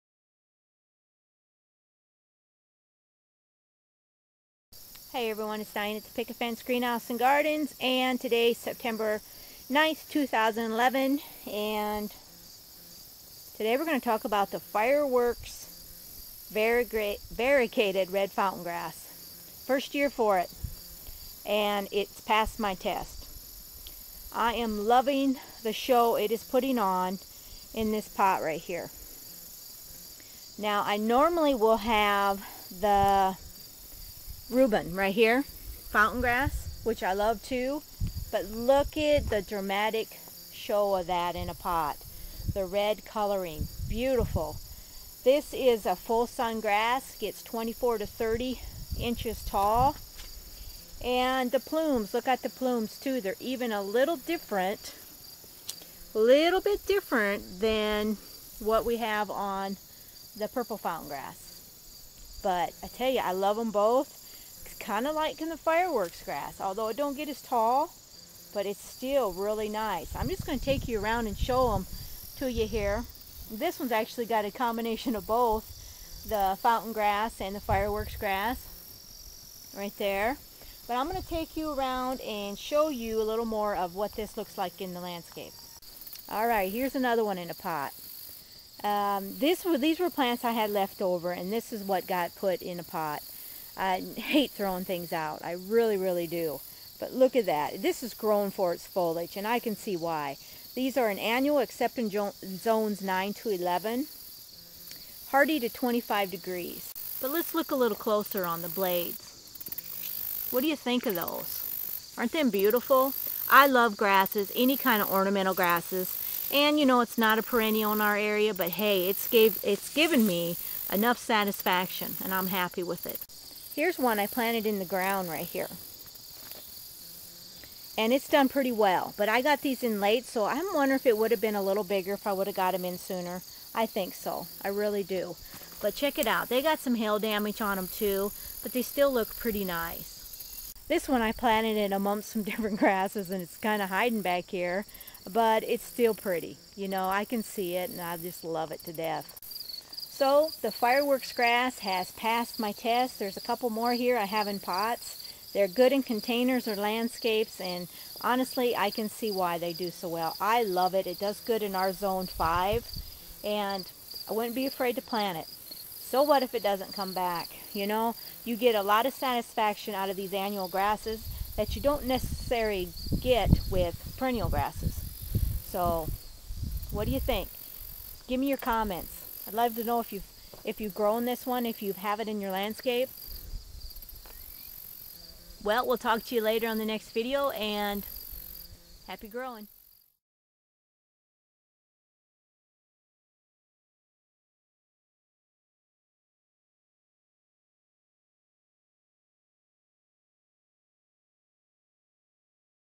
How To Grow Pennisetum Variegated Red Fountain Grass Fireworks- Dramatic-Intense Color All Season
Ebay Store a@

My personal testimony of the fireworks grass is this is a outstanding , dramatic intense show in containers or the landscape.. It is just beautiful and easy to grow too!

Pennisetum Variegated Red Fountain Grass Fireworks

Wow , if this grass turns out like the photo , it will be a hit in the gardening world.

Ornamental' s grasses is something everyone should add to their landscape, because of the easy care and neglect most can handle through the season.

Features

• Deadheading Not Necessary
• Foliage Interest
• Grass
• Landscape Plant
• Low Maintenance
• New

Multicolored foliage and pink plumes, sounds like a great combination of color.

Grows 24-30 inches tall,
Thriller -- Used either in the back or middle of a container. Usually a taller item that adds height and drama to the container.

Treat as a annual except  for  zones 9-11, as Hardiness is to 25 degrees. Likes a sun location  , as most grasses prefer.

Plant this beautiful ornamental grass in Landscapes,  containers and Great in beds, along walkways, and on patios.

This fireworks grass will be new for 2011, really can't wait to see it perform and grow.

Write  your comments below , what you think , thoughts and opinions would love to hear from you.